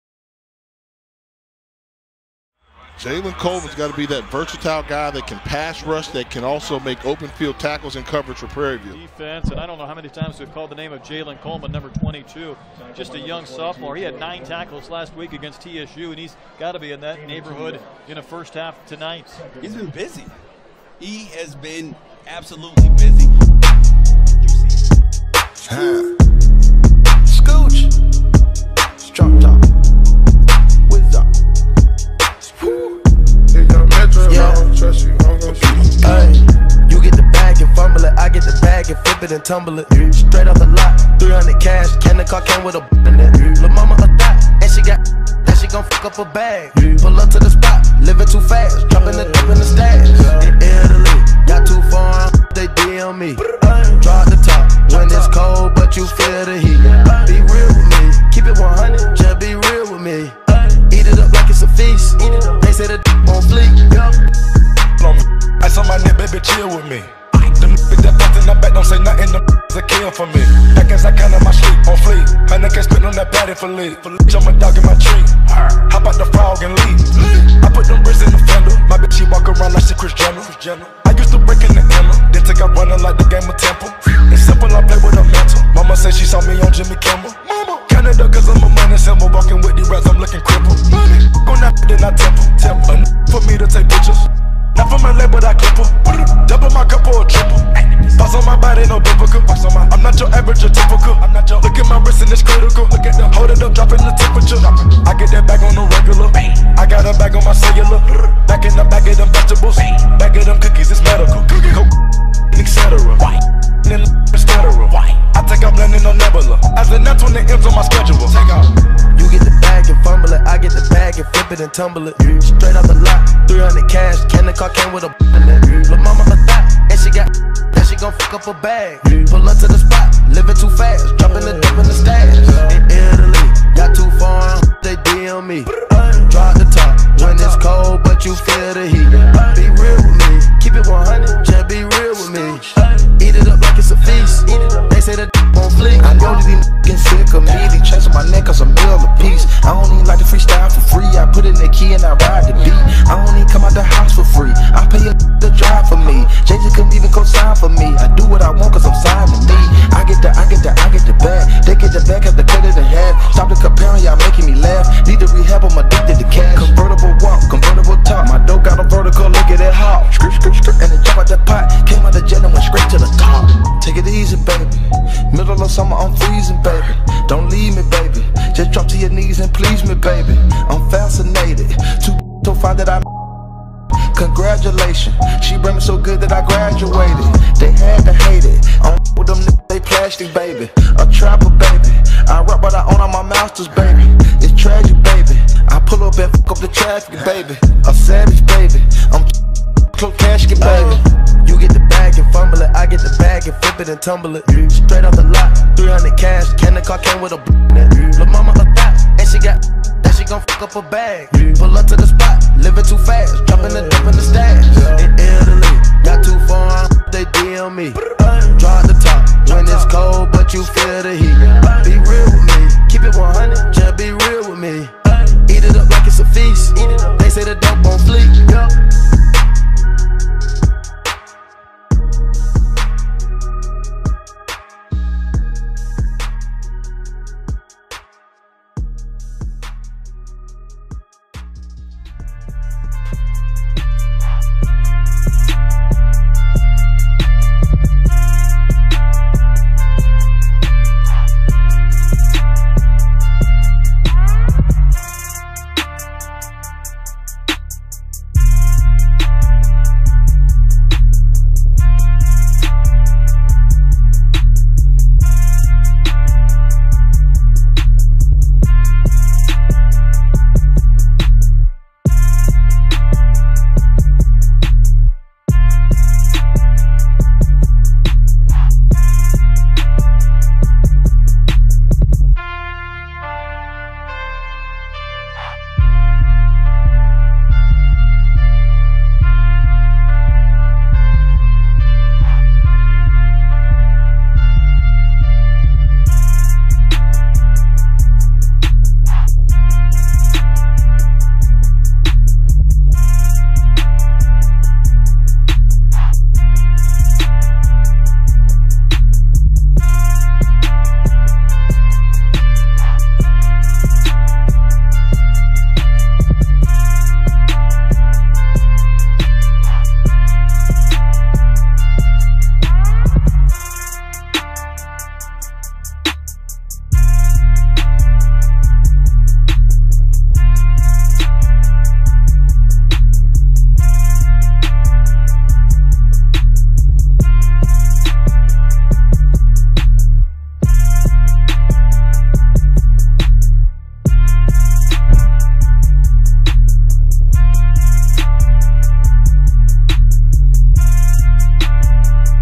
Jalan Coleman || Ultimate 2015-2017 Highlights || Prairie View A&M
Jalan Coleman is a versatile playmaker that can play multiple positions, from defensive end, middle linebacker, outside linebacker, and nickel.